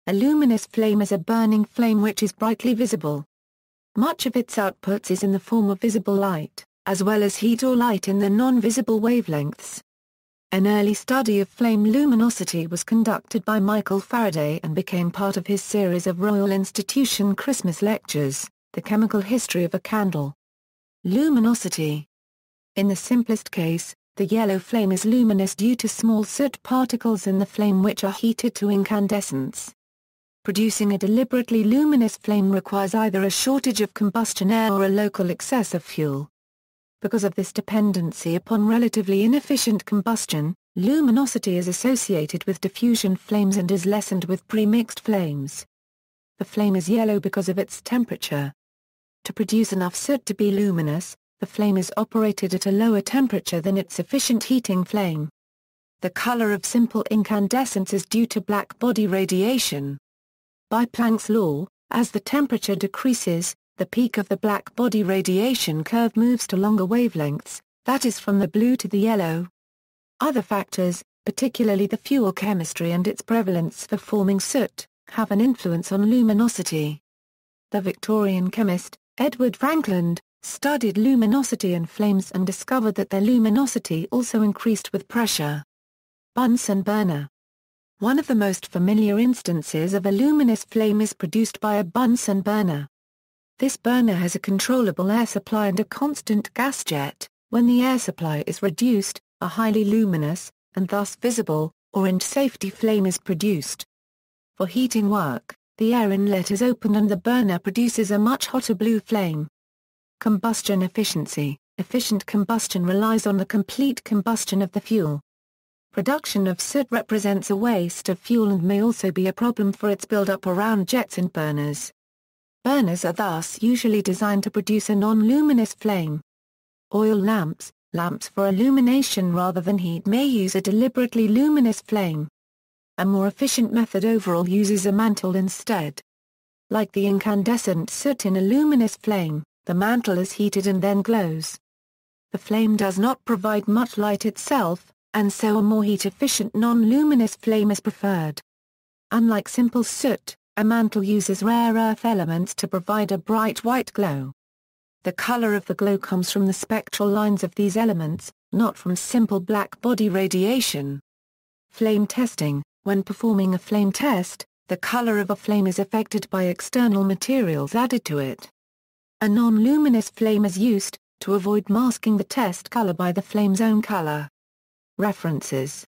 Luminous flame
A luminous flame is a burning flame which is brightly visible. Much of its output is in the form of visible light, as well as heat or light in the non-visible wavelengths.
An early study of flame luminosity was conducted by Michael Faraday and became part of his series of Royal Institution Christmas Lectures, The Chemical History of a Candle.